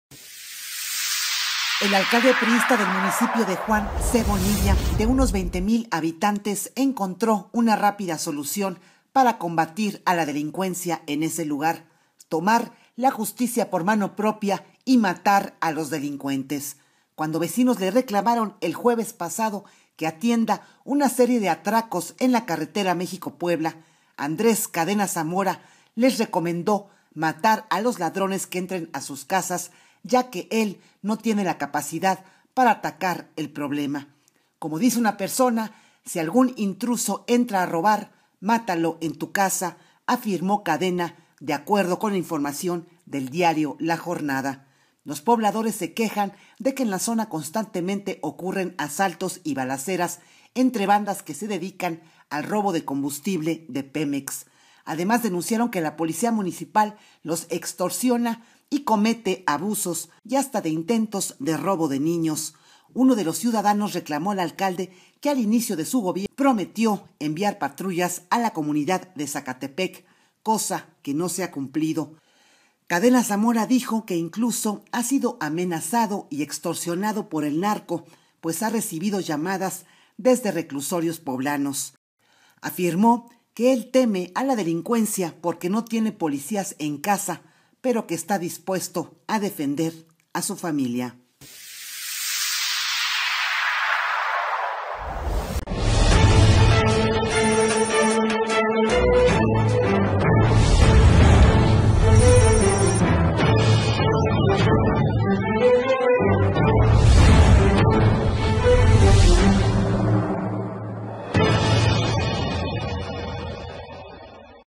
Alcalde priista en Puebla pide a ciudadanos matar a ladrones
El alcalde priista del municipio de Juan C. Bonilla, Andrés Cadena Zamora, encontró una rápida solución para combatir a la delincuencia en ese lugar: tomar la justicia por mano propia y matar a los delincuentes.